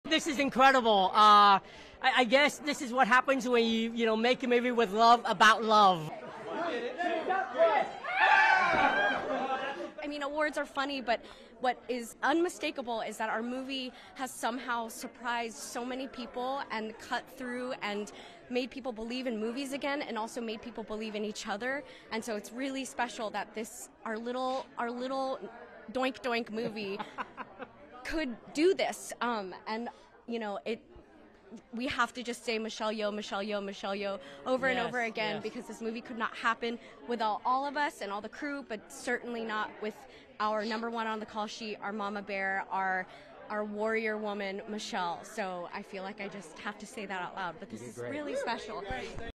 Stars of "Everything Everywhere All at Once" elated by winning Gotham Awards for best feature, outst
ASSOCIATED PRESS
New York – 28 November 2022
1. SOUNDBITE (English) Ke Huy Quan, actor - after winning best supporting performance:
"This is incredible. I guess this is what happens when you, you know, make a movie with love about love."
2. Cast and crew of "Everything Everywhere All at Once" jump in the air ++PART OVERLAYS SHOT 3++
3. SOUNDBITE (English) Stephanie Hsu, actor - after "Everything Everywhere All at Once" wins best feature:
“Awards are funny, but what is unmistakable is that our movie has somehow surprised so many people and cut through and made people believe in movies again and also made people believe in each other. And so, it's really special that this - our little, our little doink-doink movie - could do this and, you know it. We have to just say Michelle Yeoh, Michelle Yeoh, Michelle Yeoh over and over again, because this movie could not happen without all of us and all the crew, but certainly not without our number one on the call sheet, our mama bear, our warrior woman, Michelle. So, I feel like I just have to say that out loud, but this is really special."
STORYLINE: